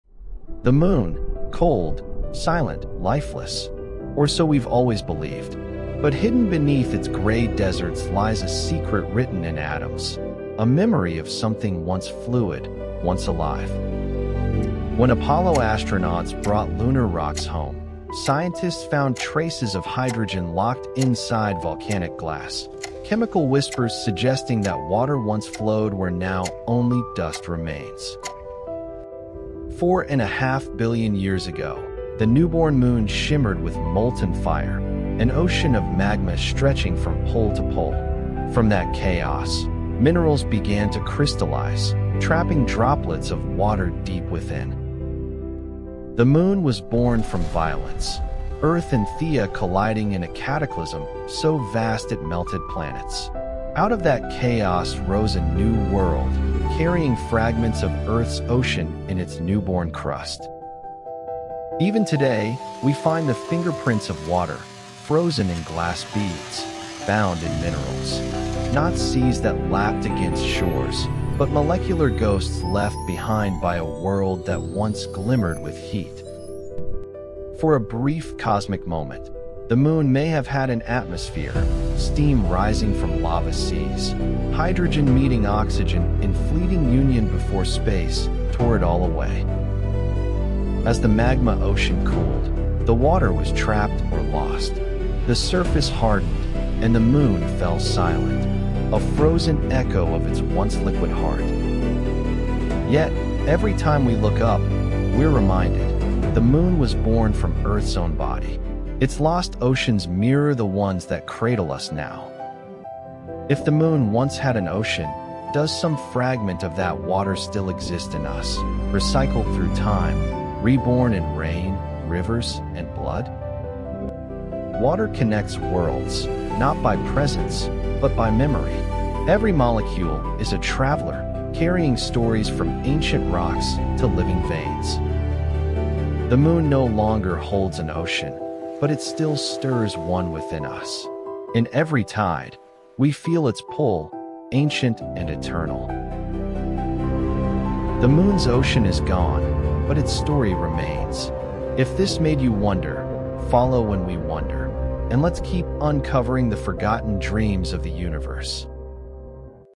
The Moon Once Had an Ocean | When We Wonder
We look up at the Moon and see silence — grey dust, dead craters, and eternal night.
But hidden deep within its stones lies an astonishing truth: the Moon once had an ocean.

Billions of years ago, it glowed with molten seas of fire. Steam rose from lava plains, and a thin, fleeting atmosphere wrapped its body. In those early moments, water — the molecule of life — shimmered within its molten heart.

When the surface cooled, that ocean vanished… yet traces remained, locked in glass, carried across time as a memory of Earth’s own beginnings.

This film blends astronomy, geology, and poetic imagination to explore the Moon’s lost ocean — and the strange way it still lives within us.
Because when we gaze up at the night sky, we’re not just seeing another world — we’re looking at our planet’s first mirror.

If this story moved you, follow When We Wonder — and keep exploring the mysteries that connect us to the cosmos.